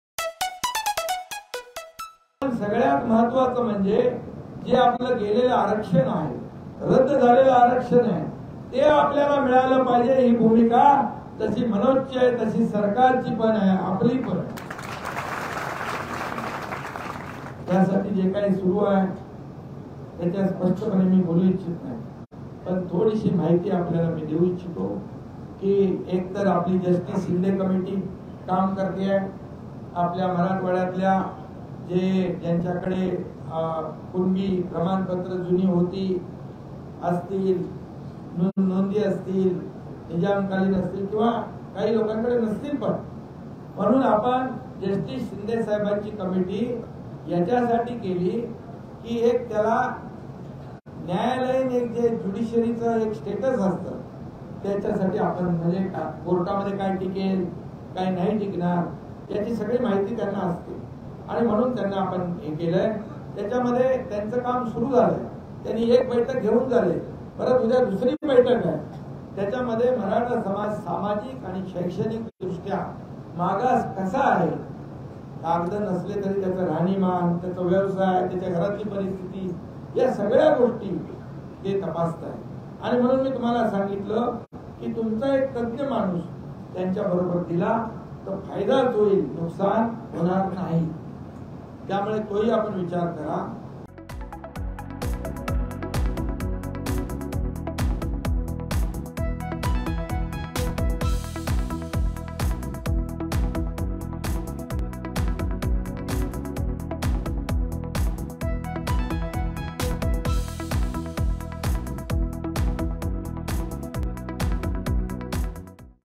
Eknath Shinde यांना मराठा आरक्षण देण्याबाबतची सांगितली प्रक्रिया | Maratha Reservation
मनोज जरांगे यांच्या उपोषणस्थळी जाऊन एकनाथ शिंदे यांनी त्यांची भेट घेतली. त्यांच्या भेटीनंतर मनोज जरांगे यांनी उपोषण सोडले